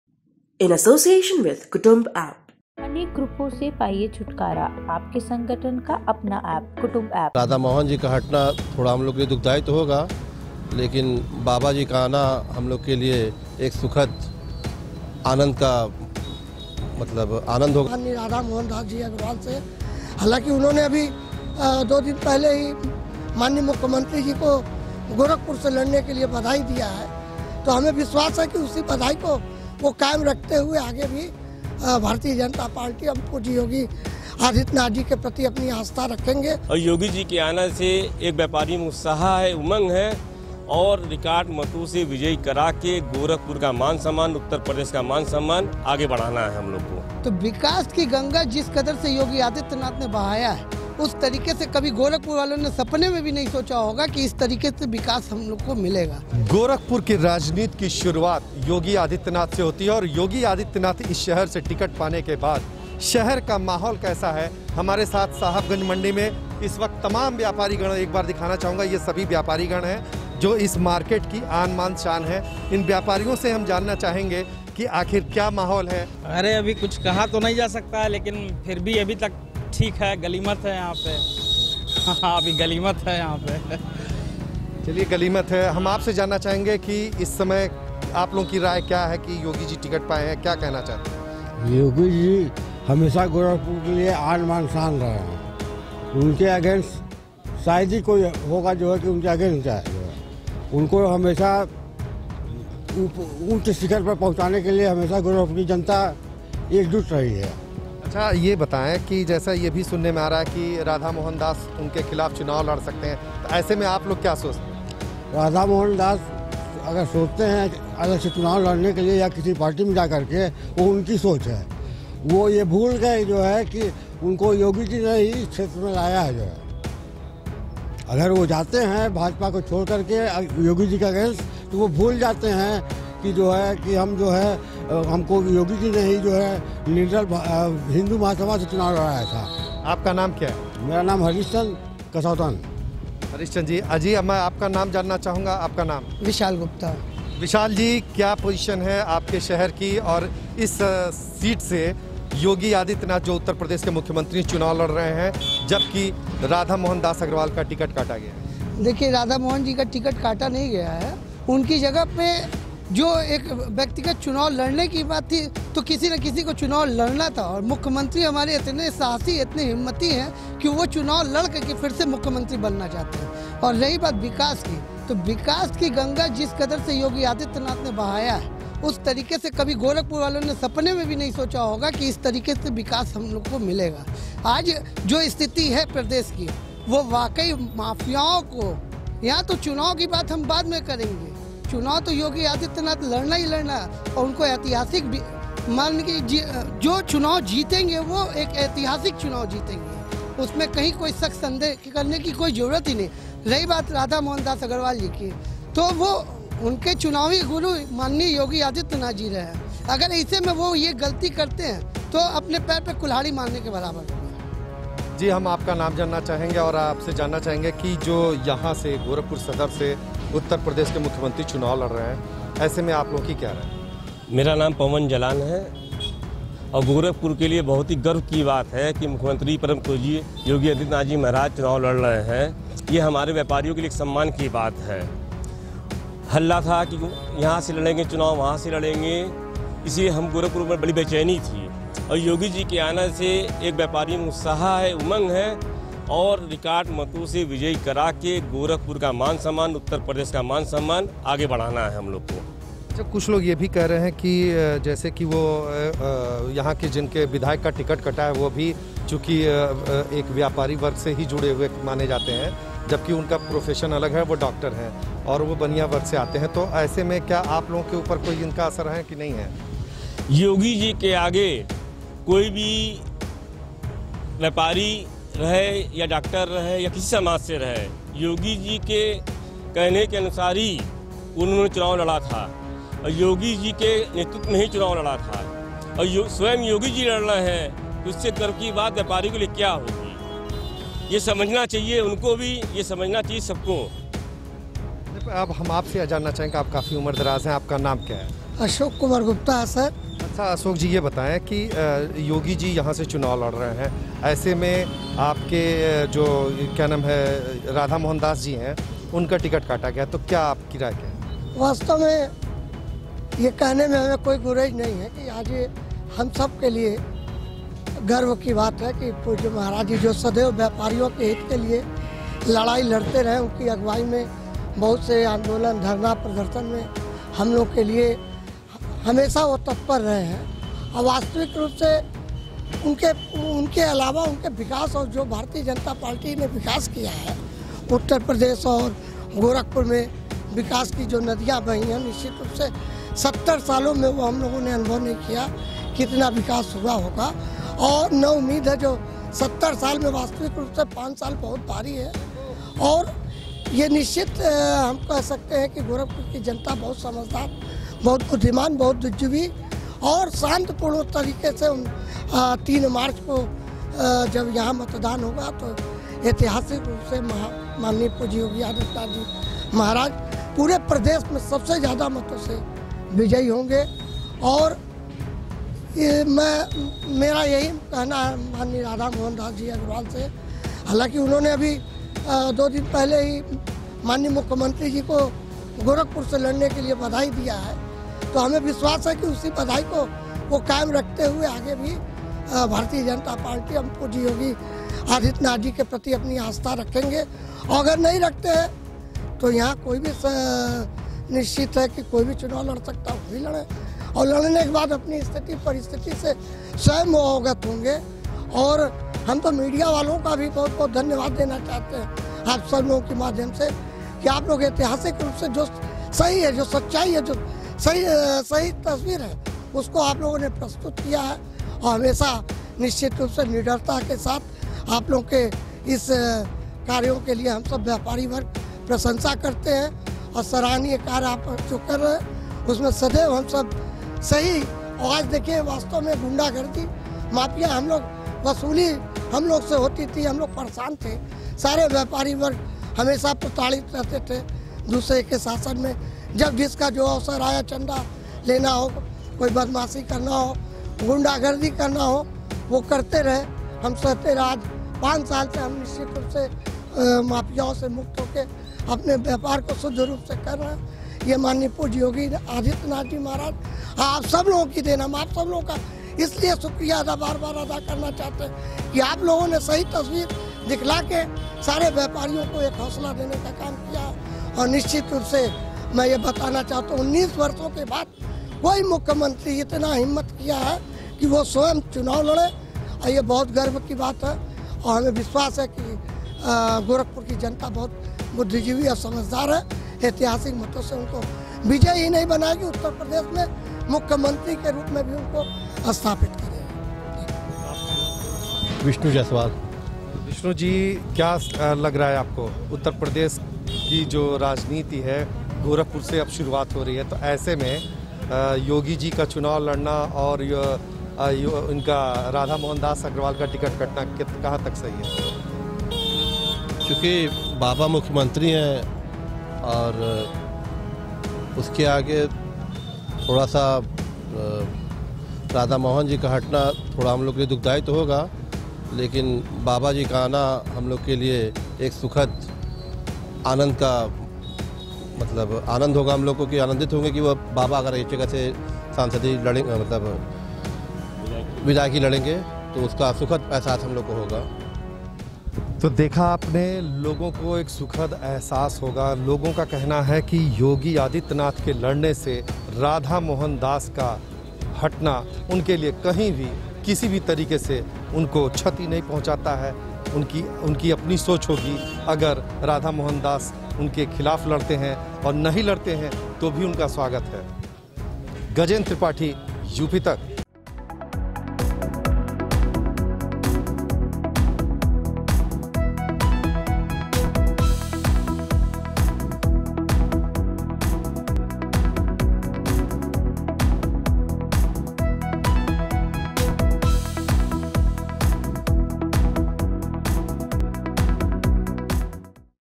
Gorakhpur Ground Report: योगी के आने और राधा मोहन अग्रवाल का टिकट कटने पर क्या बोला वैश्य समाज?
भाजपा के विधायक राधा मोहन अग्रवाल का टिकट कटने पर क्या बोले गोरखपुर के वैश्य समाज के लोग?